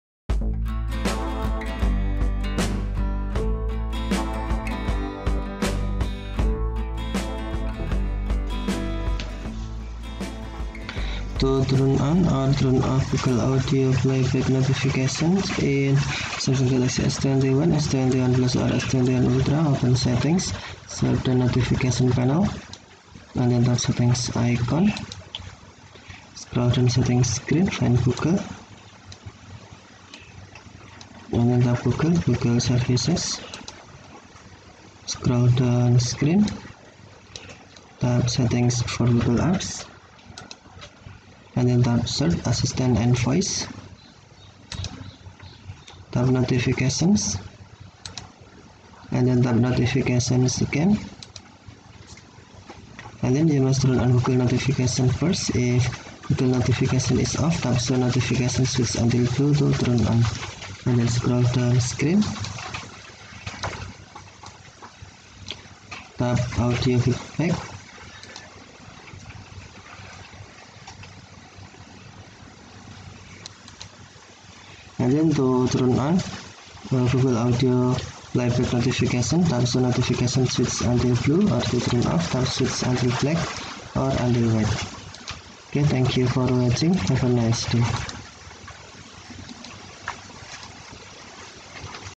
How to turn on or off Google Audio playback notification in Samsung Galaxy S21, S21Plus, or Ultra
This video show How to turn on or off Google Audio playback notification in Samsung Galaxy S21/S21+/S21 Ultra.  In this tutorial I use Samsung Galaxy S21+ SM-G996B/DS International version with Android 11